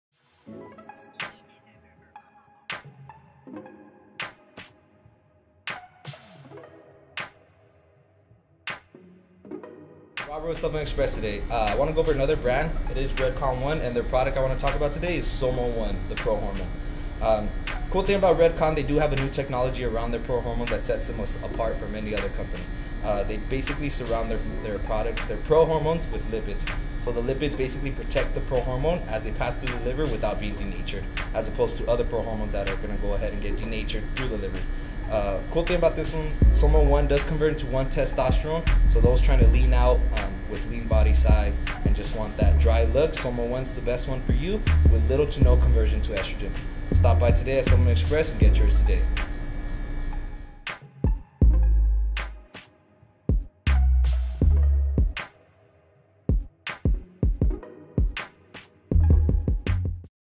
Weekly Review Redcon1 Somal 1
This week we review Redcon1 with their awesome Somal 1 product. Find out how this product helps you get the results your looking for.